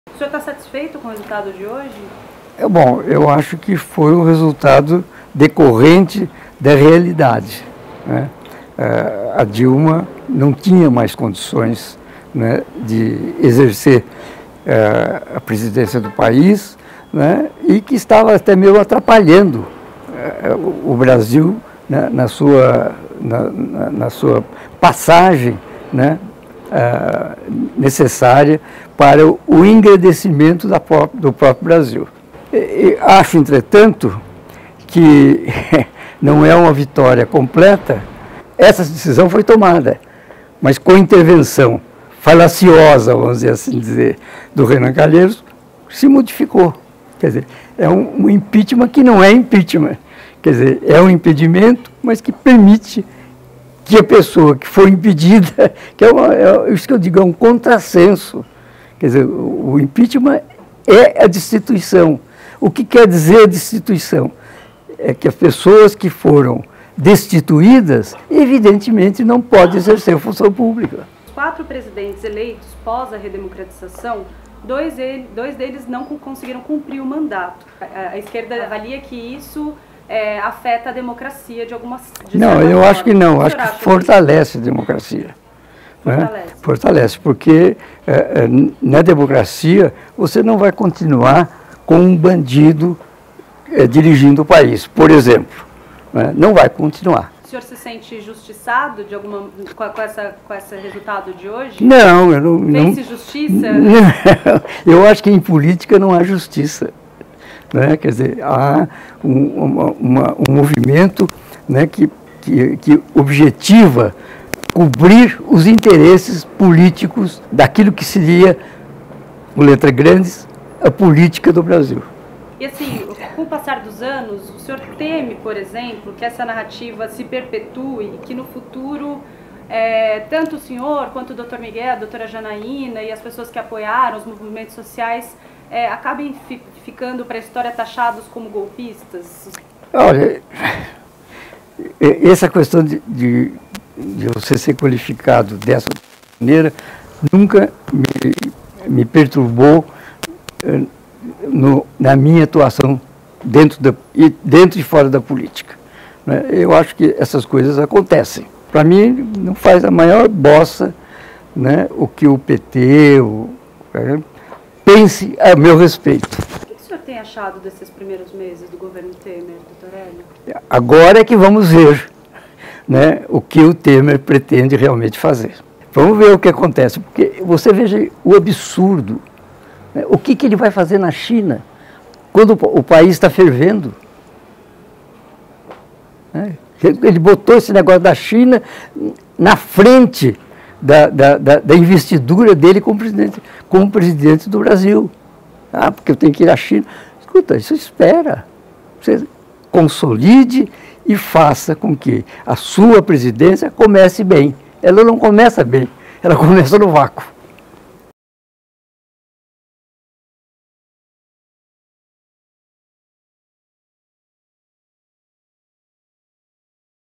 O homem que ajudou a derrubar Dilma
O jurista Hélio Bicudo, ex-filiado ao Partido dos Trabalhadores, é co-autor do pedido de afastamento da ex-presidente. Ao emprestar seu nome à petição, conferiu o prestígio necessário para que o julgamento tivesse o desfecho de hoje.